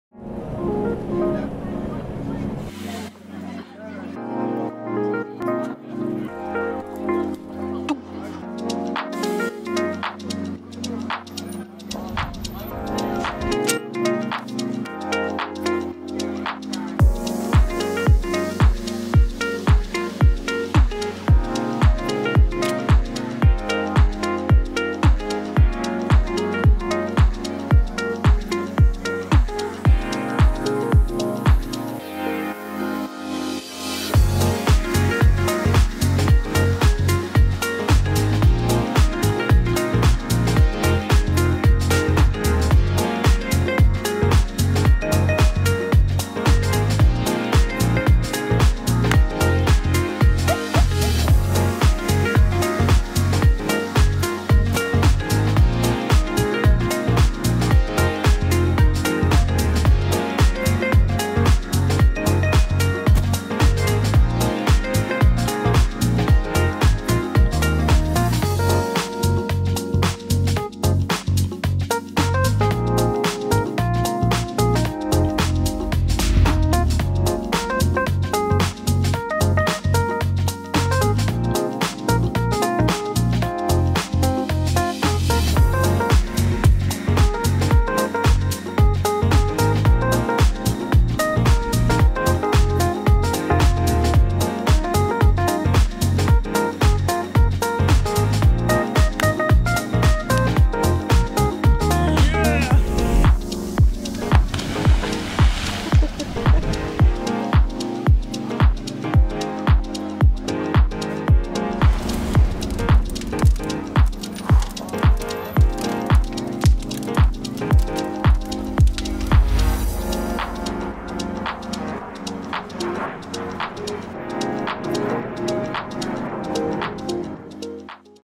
Climbing Tromsø | Tromsø Klatresenter
We headed to the artic capital to visit a friend and to climb some new walls. Tromsø klatresenter had a lot of different walls and great bouldring. Most of our trip was spendt in the climbing gym, but we also got to go swimming in the cold artic water and walk around in the city.